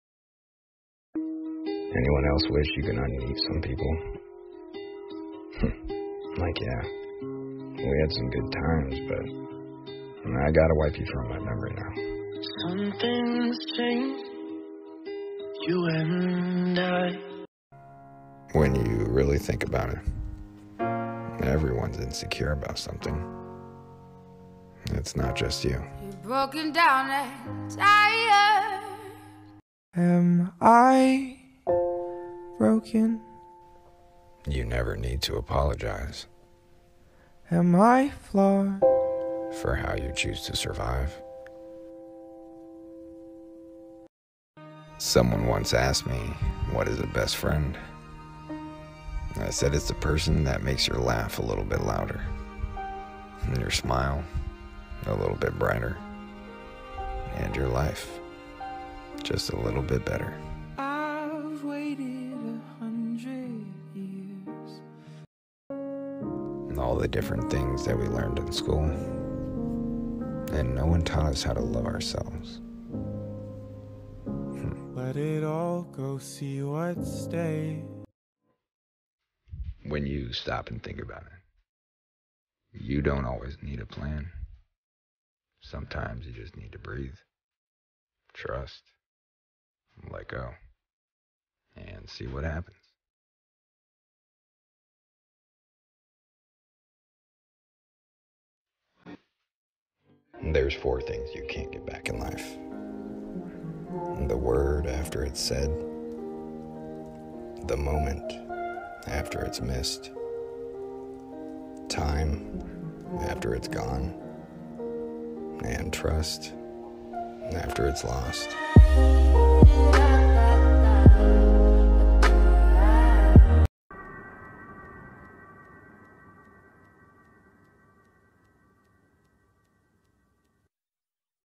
We had good times but I gotta wipe you from my memory now.
Anyone else wishes you can unmeet some people?
Like yeah...
We had good times but,
I gotta wipe you from my memory now.

When you really think about it,
Everyone's insecure about something.
It's not just you.

You never need to apologize,
For how you choose to survive.

Someone once asked me,
What is a best friend?
I said it's a person that makes you laugh a little bit louder,
Your smile,
A little bit brighter,
And your life,
Just a little bit better.

All the different things that we learned in school,
And no one taught us how to love ourselves.

When you stop and think about it,
You don't always need to plan.
Sometimes you just need to breathe,
Trust,
Let go,
And see what happens.

There're 4 things you can't get back in life:
The world after it's said,
The moment
After it's missed,
Time
After it's gone,
 And trust
After it's lost.